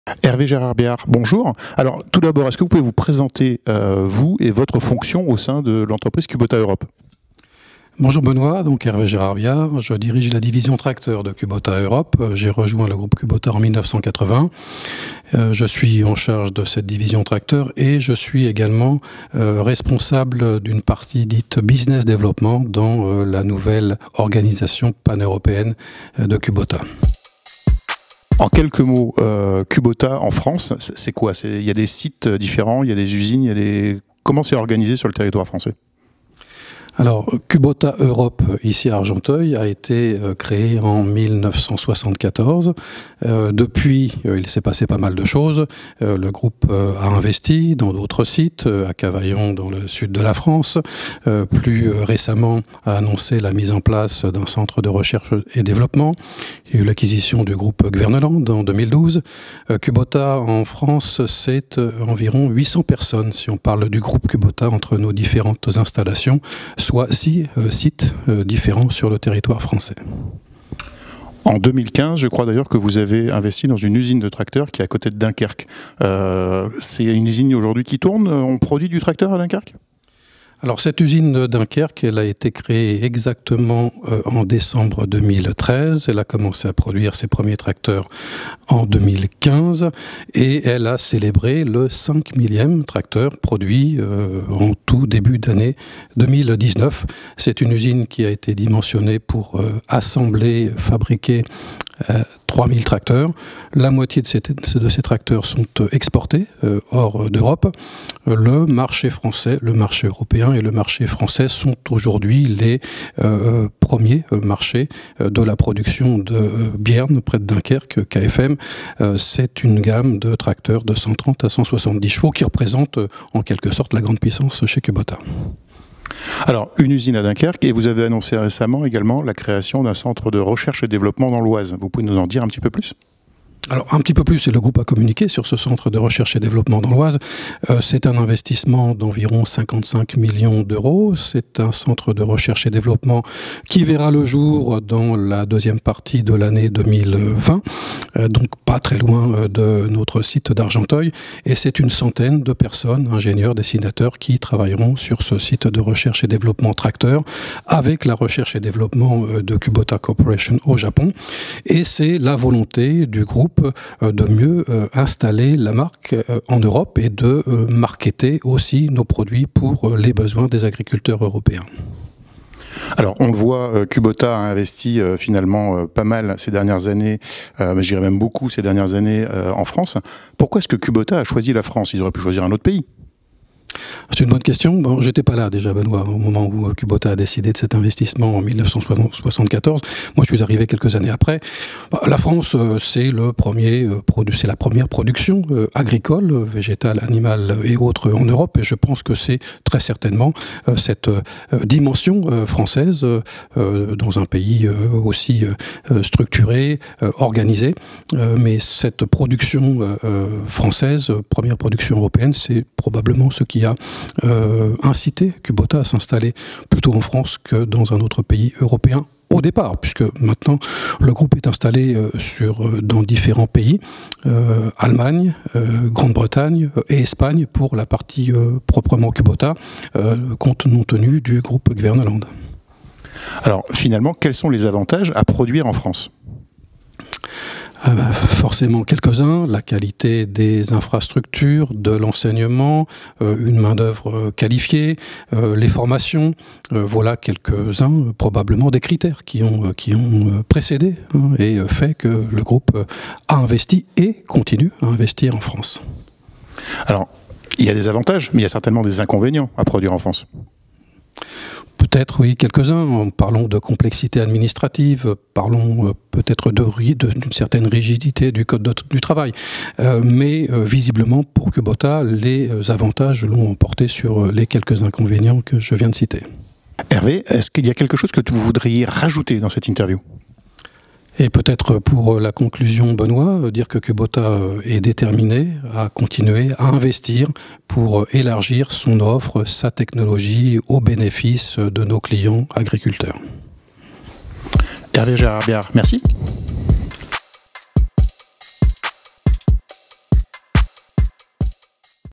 Kubota, Hervé Gérard-Biard : « Déjà 5 000 tracteurs produits en France ! »
Dans une interview exclusive, Hervé Gérard-Biard, le patron de Kubota tracteur en Europe s’exprime sur la montée en puissance des tracteurs « made in France » ainsi que sur les avantages et inconvénients de produire des tracteurs en France. Il évoque également la stratégie de développement de Kubota sur le Vieux continent.